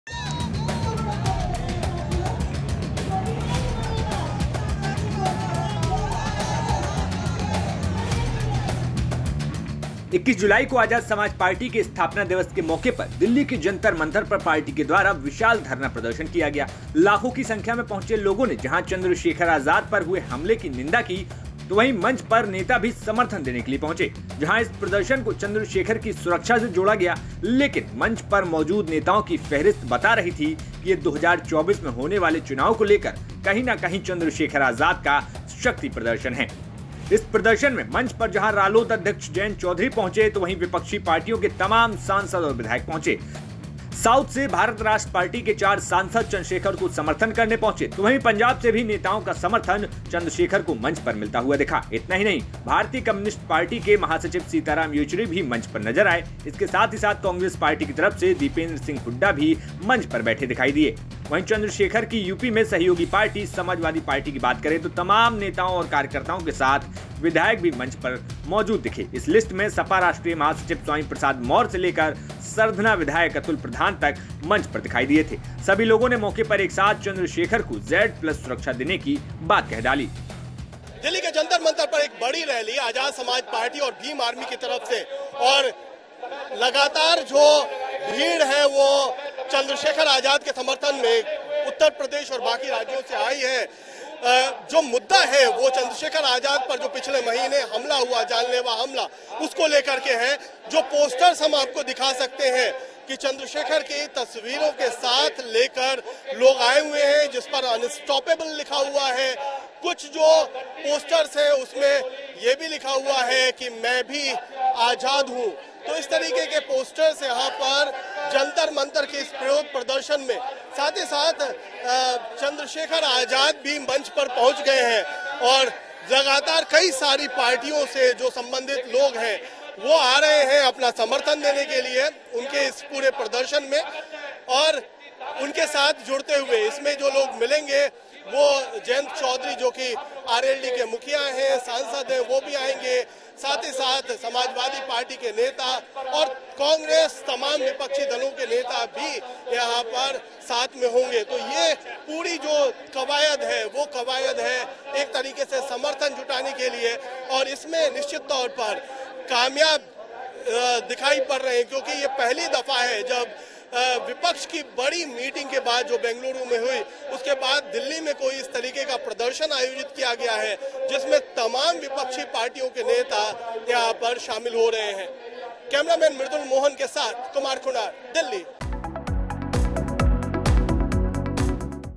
Chandrashekhar Azad : बैंगलोर में गठबंधन ने नहीं बुलाया? चंद्रशेखर ने जंतर मंतर पर दिखाया ये दम...
जंतर मंतर पर चंद्रशेखर आजाद का धरना प्रदर्शन. कर दी ये बड़ी बात...

यूपी की हर खबर अब देखें ही नहीं पढ़ें भी, लॉग-ऑन करें पर..